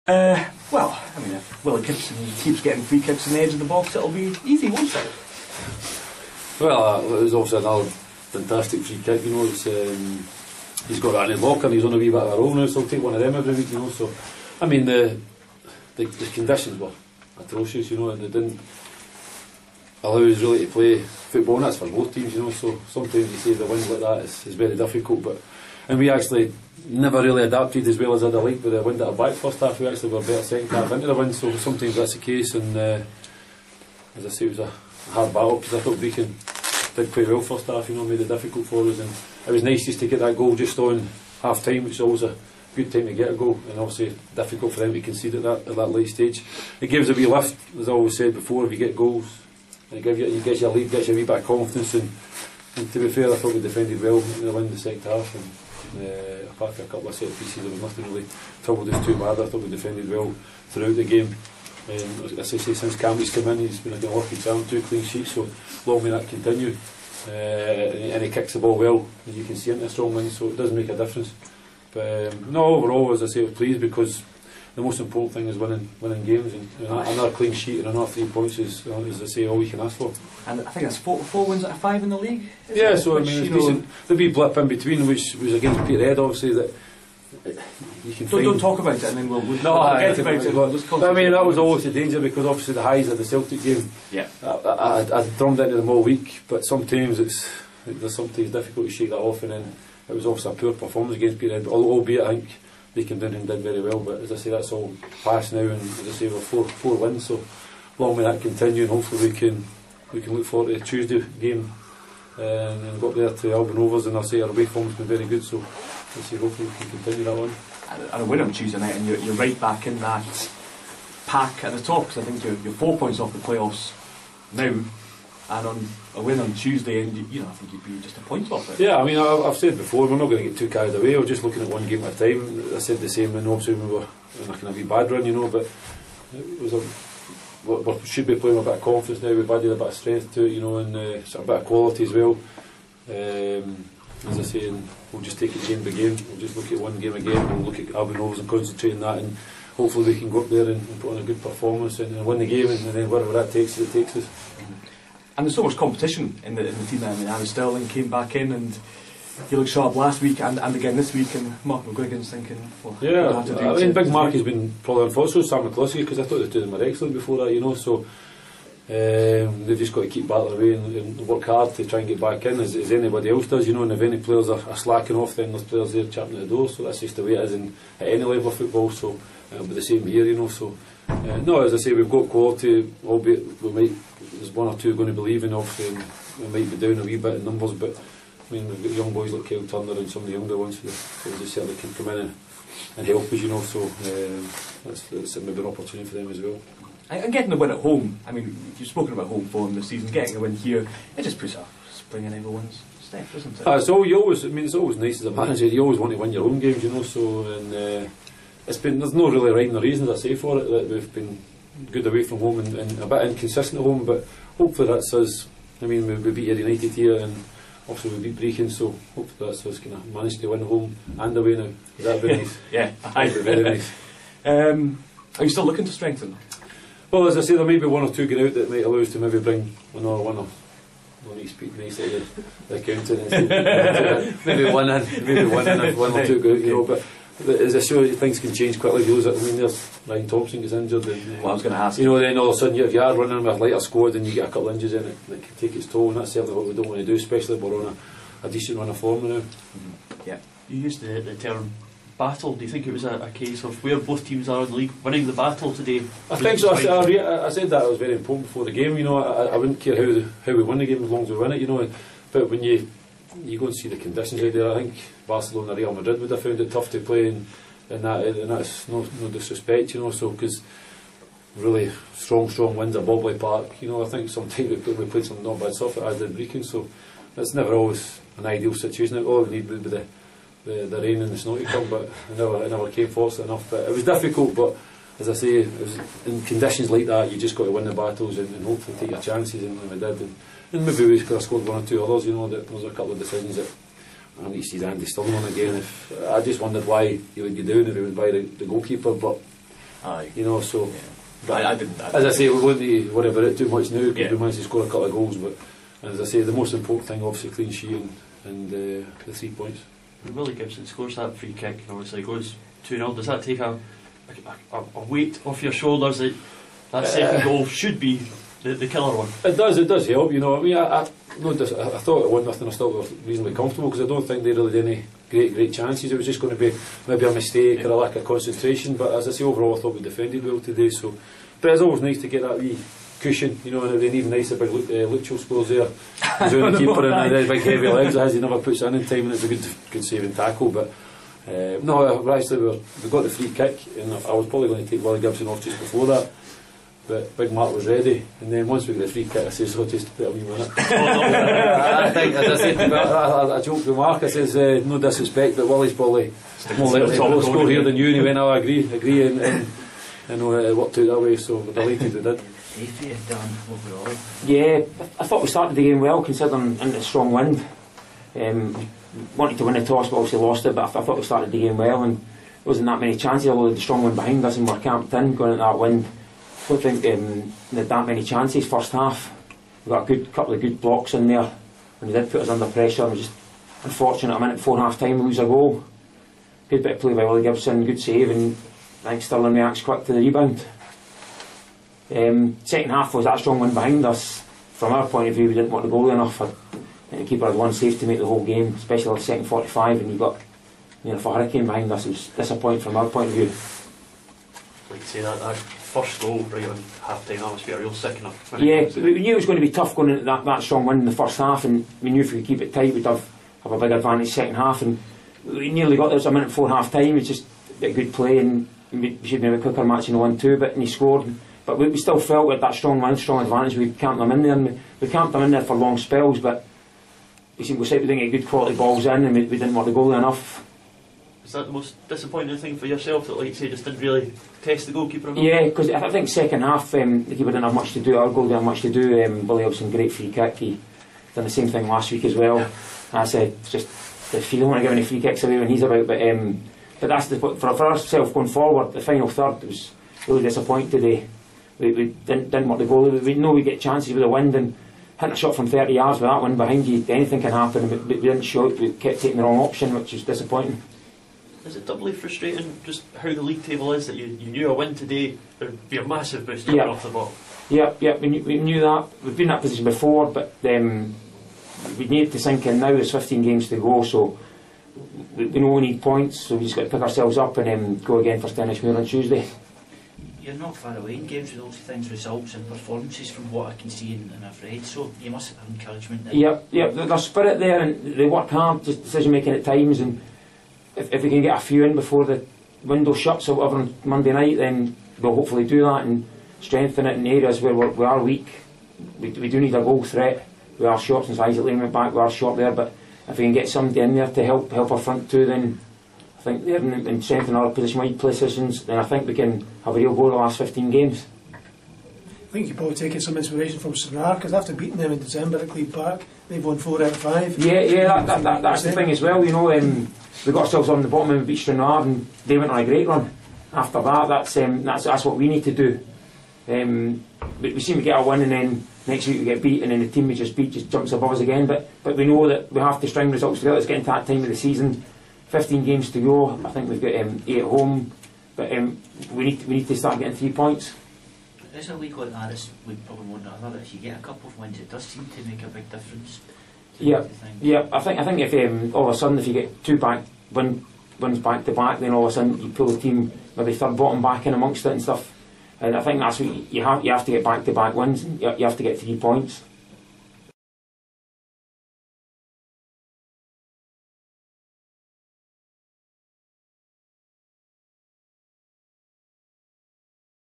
Stranraer and Brechin City Manager Interviews 30/01/2016
Stranraer Manager Brian Reid and Brechin City Manager Darren Dods give their reactions to today's game.